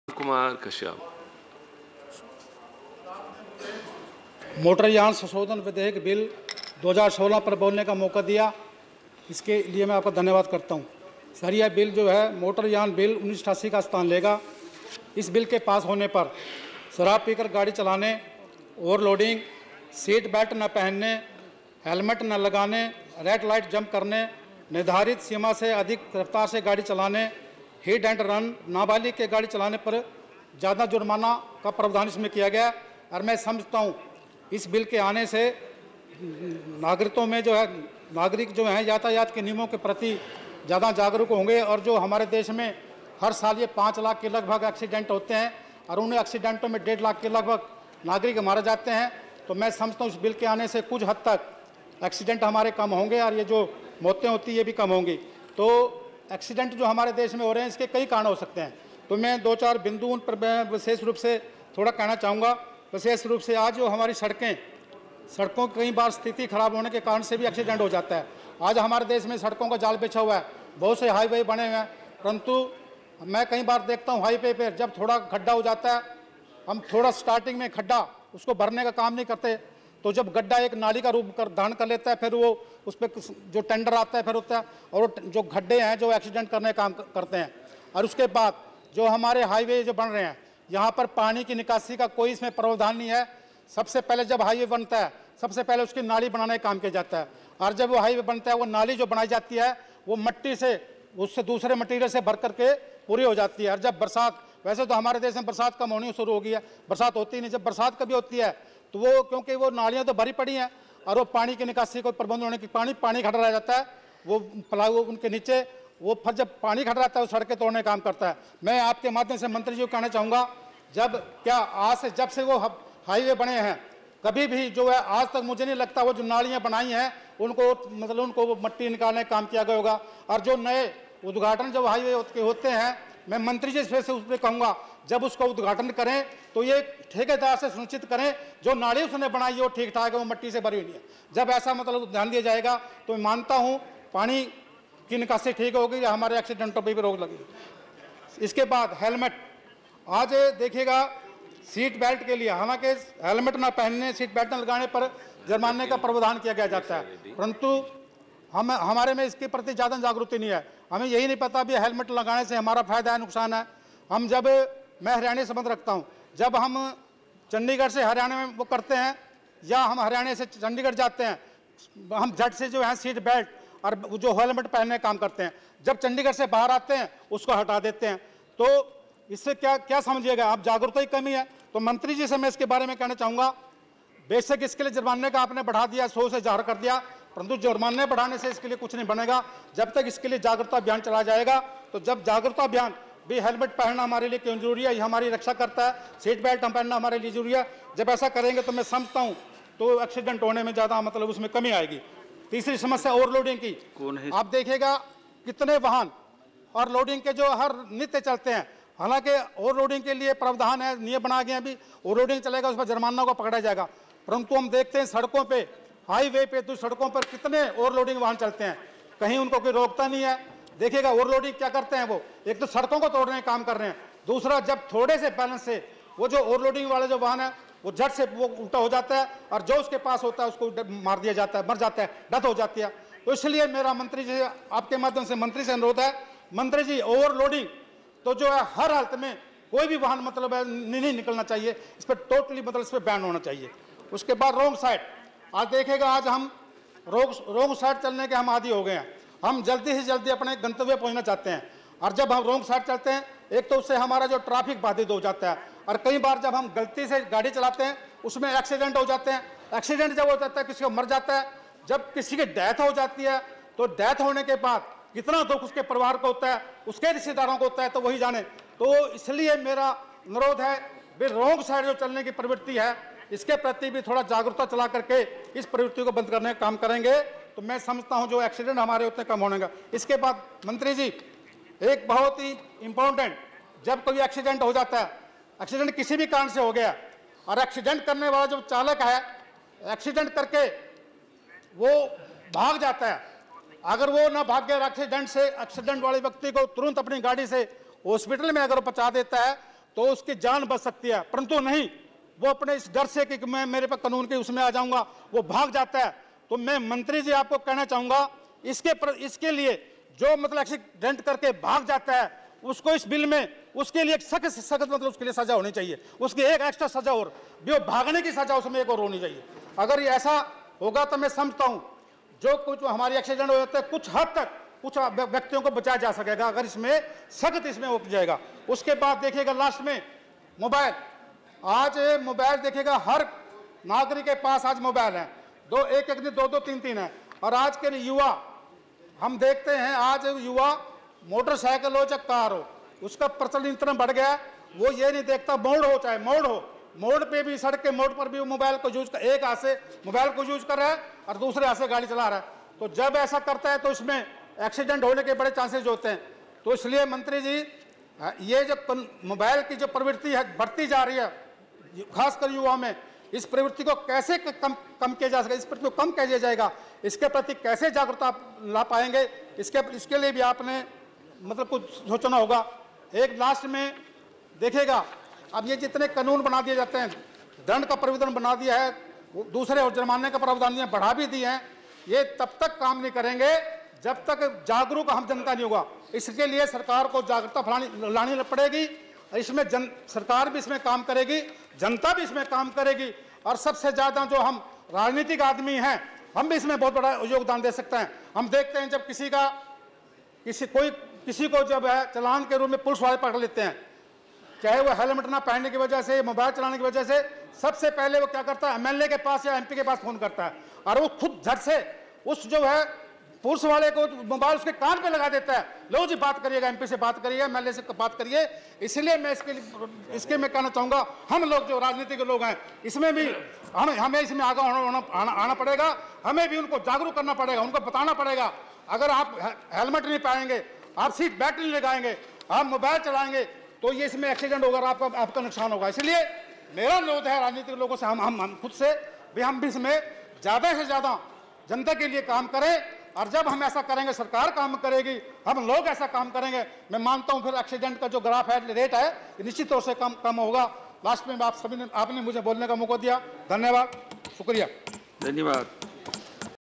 Sh. Ram Kumar Kashyap's remarks | The Motor Vehicles (Amendment) Bill, 2017
Rajya Sabha Session - 246 | Monsoon Session | July 23, 2018